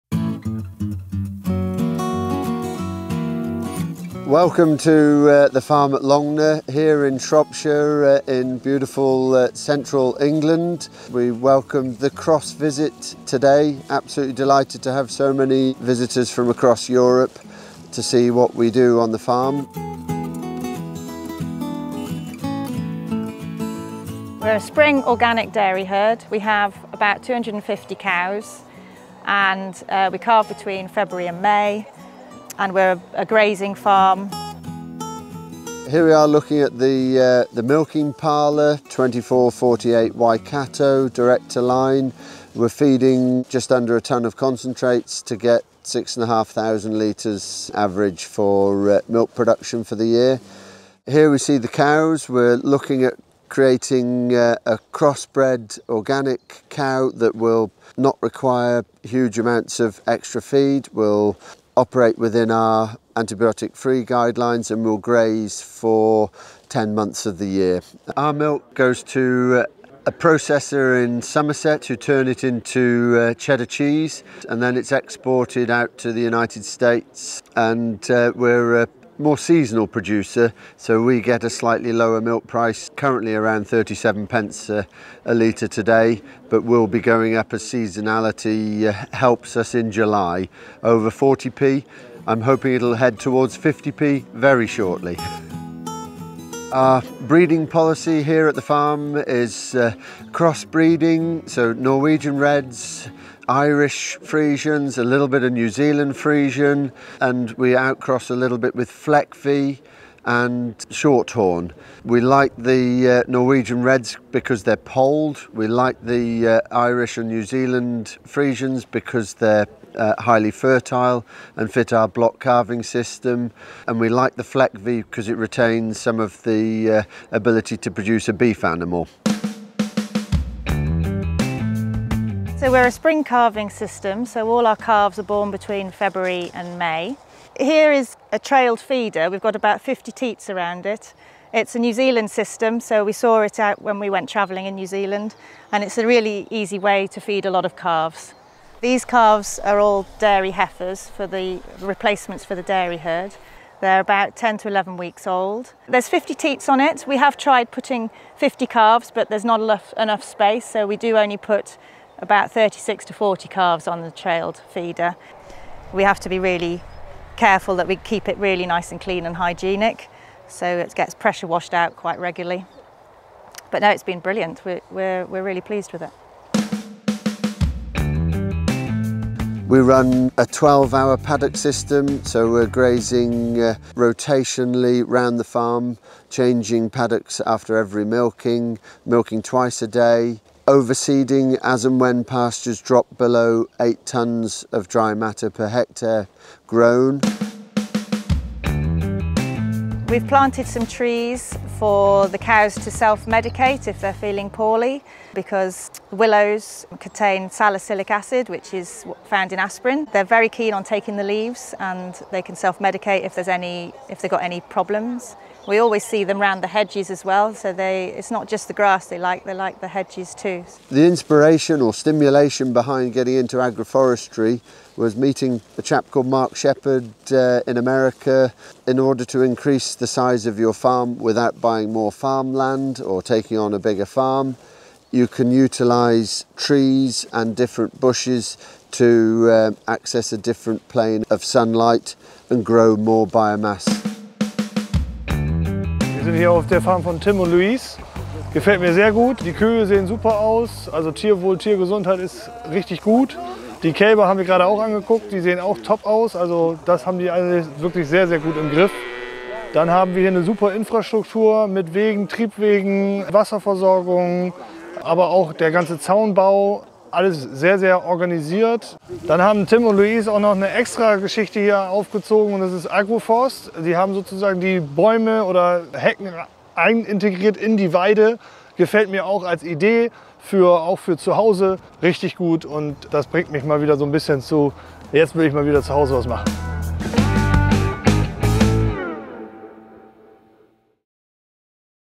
Exploring Agroforestry in Organic Dairy – NEFERTITI Cross Visit at Tim and Louise Downes' farm
00:00 Introduction
00:34 Milk performance and market
01:35 Breeding strategy
02:09 Outdoor calf feeding
02:57 Pasture Management
03:17 Agroforestry
04:04 Conclusion